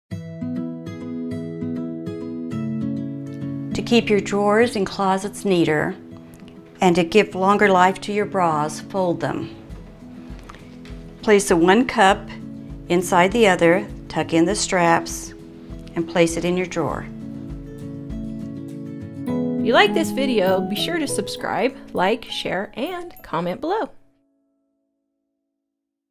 How To Fold Bras - Brassieres
Folding your bras neatly can make them last longer, help organize your dressers and closets and make everything look neater. Jill demonstrates an easy way to fold bras so they’re not strewn all over, take up less space and look neat, too!

DINING ON A DIME COOKBOOK with over 1200 recipes and tips including homemade cleaners

OUR FREE NEWSLETTER!